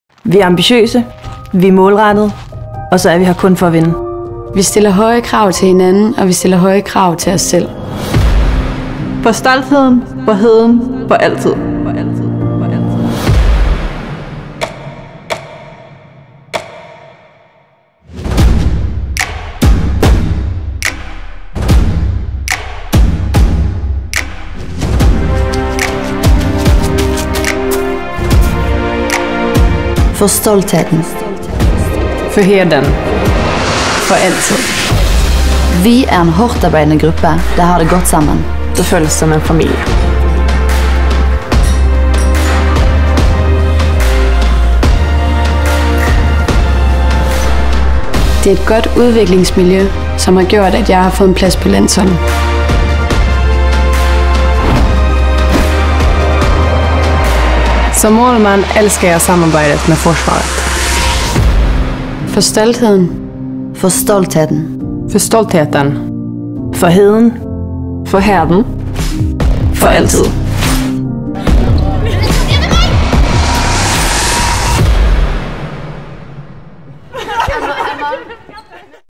Herning Ikast Håndbold - Profilfilm 2021
HIH skulle bruge en profilvideo, som kunne bruges i både hal og lounge samt til Sociale Medier.
Vi går helt tæt på nogle af stjernene i denne sportsfilm.